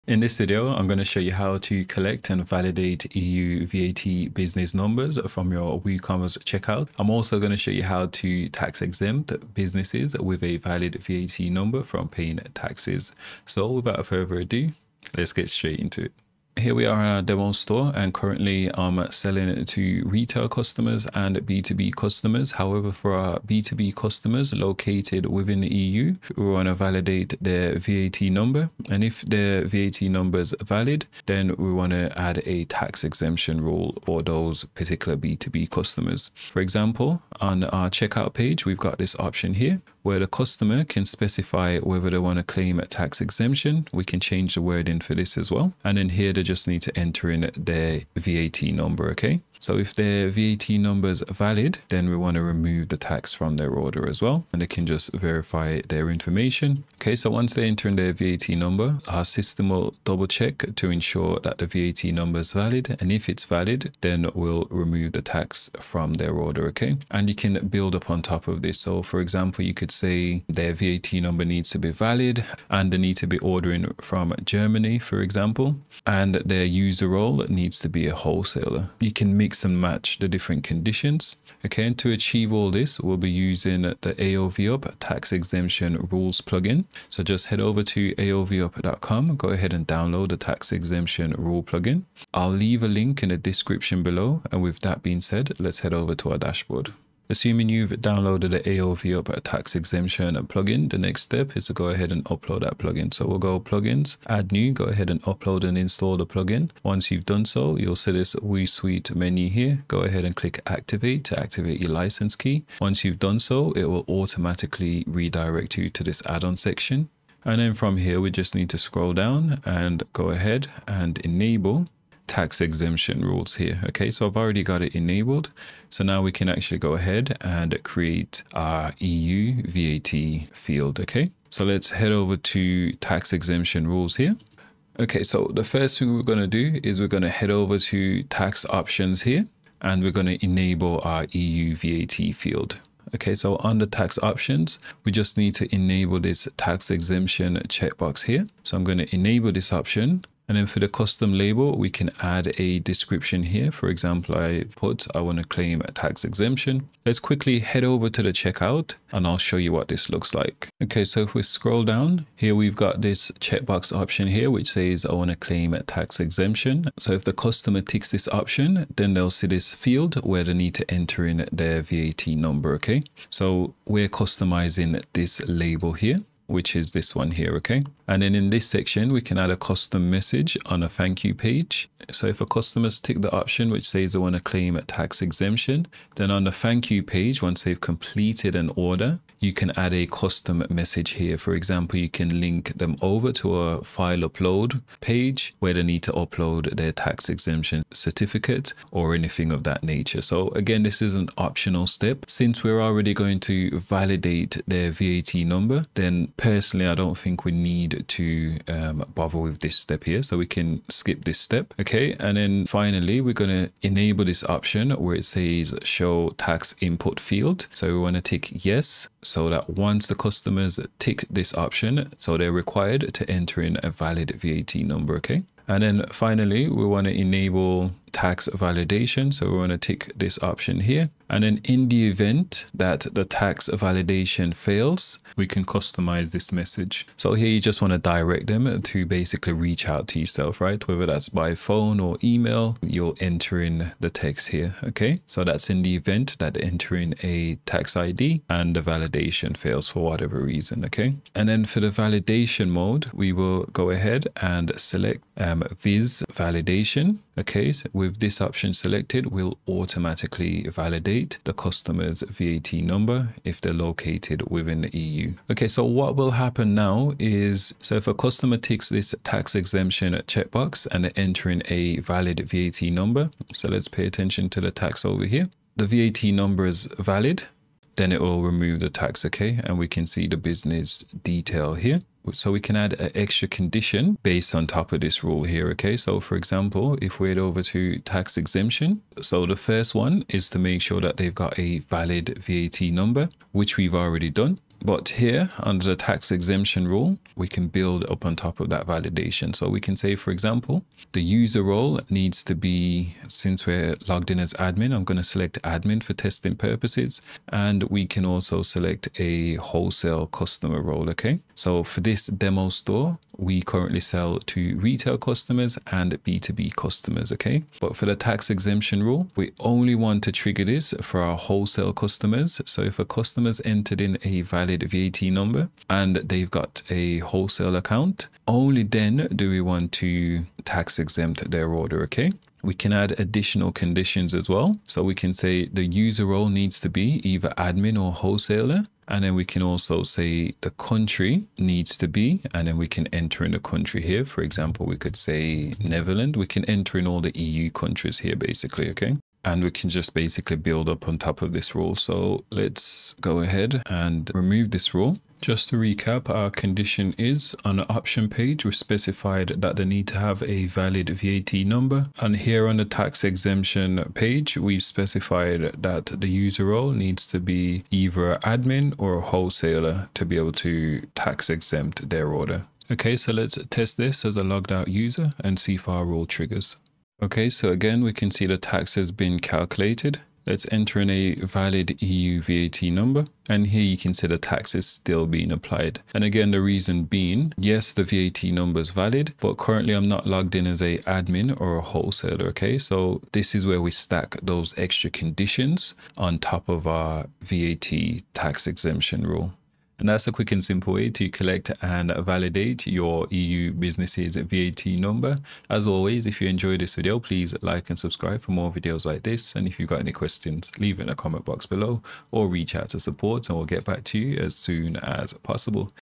WooCommerce EU VAT Number (Field & Validation)
In this video, I'll show you how to add and validate EU VAT numbers in your WooCommerce store.

00:00 Intro
00:16 Overview
01:21 Download AovUp Tax exemption
01:37 Install AovUp Tax exemption
02:05 Create EU VAT field
04:27 Test eu vat vaildation
04:42 Add condition for Tax exemption
06:17 Test as an unqualified user
06:39 Outro